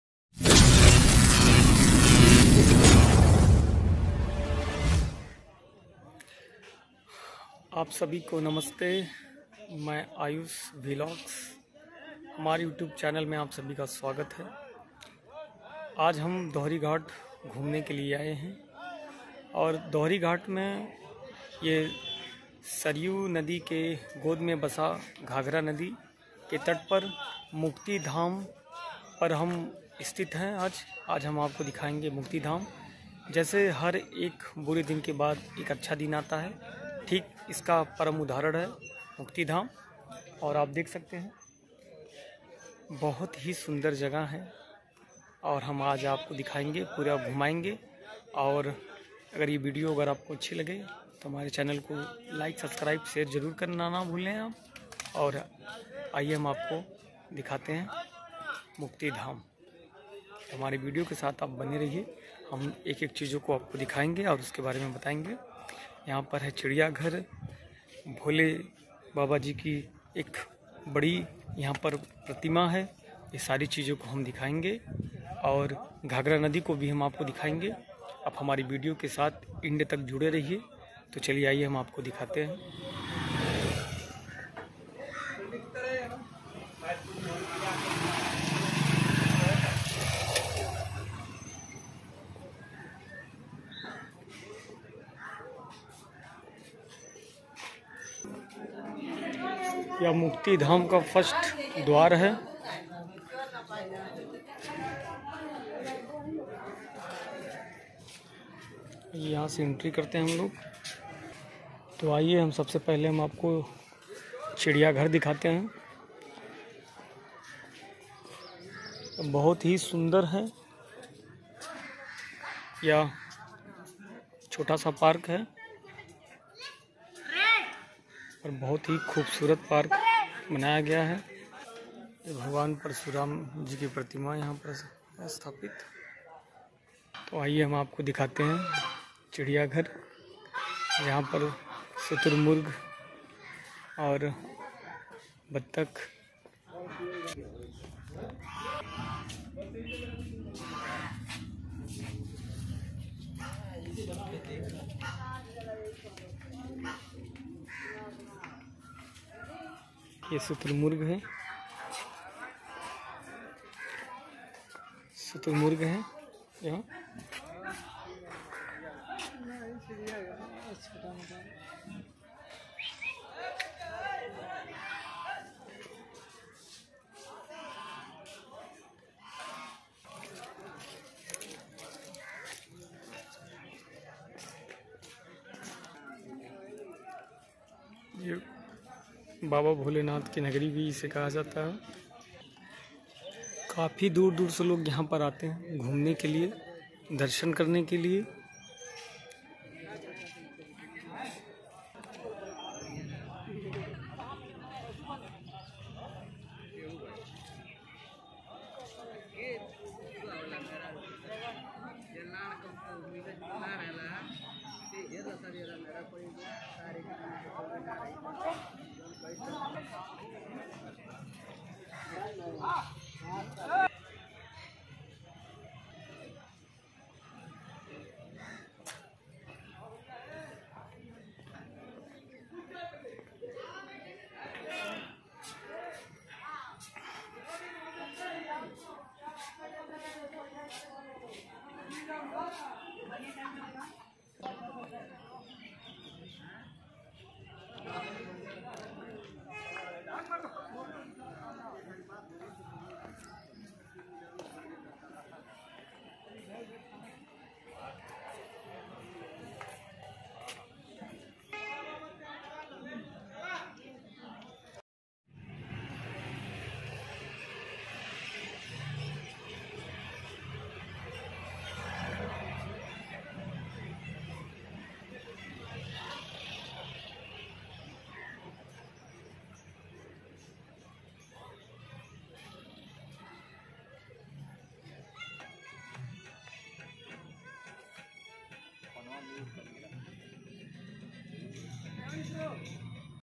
मुक्तिधाम दोहरीघाट मऊ || Muktidham Dohrighat Mau
Azamgarh
हेलो दोस्तों मैं आयुष हमारी यूट्यूब चैनल में आप सभी का स्वागत है।
धन्यवाद।।

"जब निको दिन आई है बनत न लगिहे देर" - हर बुरे दिन के बाद अच्छा दिन आता है। मुक्तिधाम इसका परम उदाहरण है। भौगोलिक दृष्टि से यह घाघरा वर्तमान सरयू नदी की गोद मे बसी है। यह स्थान श्रद्धालुओं का आकर्षण केन्द्र बना हुआ है तथा समय समय पर मेले लगते हैं। सदियों से गौरवमयी इस स्थान पर दो-हरि यानि श्री राम एंव परशुराम की मिलन स्थली जिसको आज दोहरीघाट के नाम से जानते हैं। यह बाबा भोलेनाथ की परम पवित्र काशी नगरी की परिक्षेय मे आता है इसलिए इसको काशी क्षेत्र के नाम से भी जाना जाता है। सरयू के दक्षिणी पूर्वी द्वार पर बसा यह नगर इसलिए भी धार्मिक दृष्टि से महत्वपुर्ण माना जाता है क्योंकि यहां दो किलोमीटर तक सरयू नदी उत्तर दिशा को बहती है। यहाँ बङे बङे महर्षि तपस्वी व संत वर्षों तक साधना किए हैं, ऐसे महात्माओं मे संत सिरोमणि, नागा बाबा, खाकी बाबा, मेला राम बाबा आदि। भगवान राम द्वारा स्थापित "गौरी शंकर" के भब्य मंदिर के नाते इस स्थान की चर्चा दूर-दूर तक है।

hello doston main Ayush Mere YouTube channel Mein aapka swagat hai

ayush Vlogs
From-Azamgarh Uttar Pradesh (India)